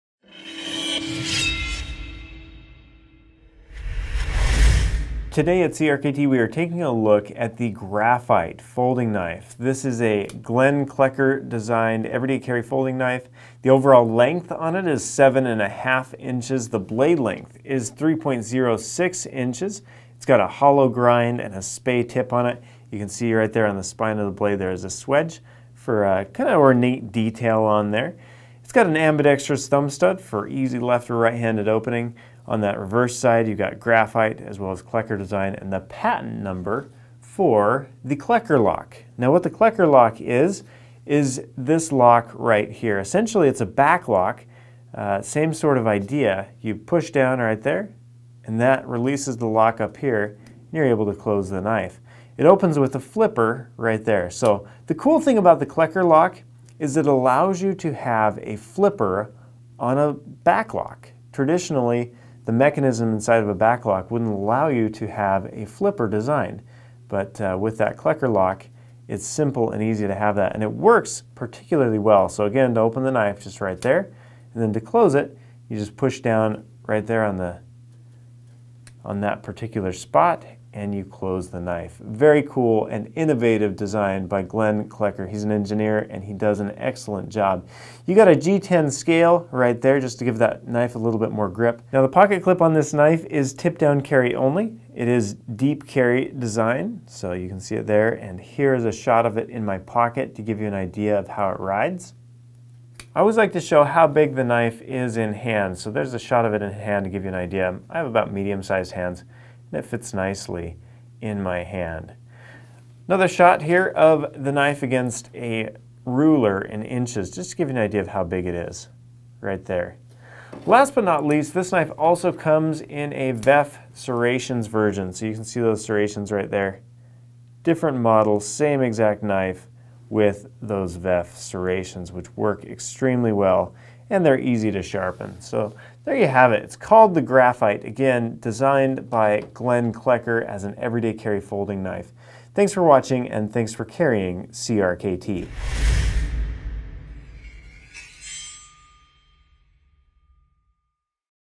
CRKT Graphite Knife | by Glenn Klecker
Looks like a sports car. Cuts through tasks like a bulldozer.

The Graphite folding knife is unique, elegant and significantly cheaper than an Italian sports car. With an open build skeletal frame and the integrated Klecker Lock for safety, Glenn Klecker's new Graphite® knife is a lightweight folder that's perfect for heavy-duty jobs. It has a modified drop point blade, that's also available with Veff Flat Top Serrations®, it cuts through anything. Pop it open and put it to work.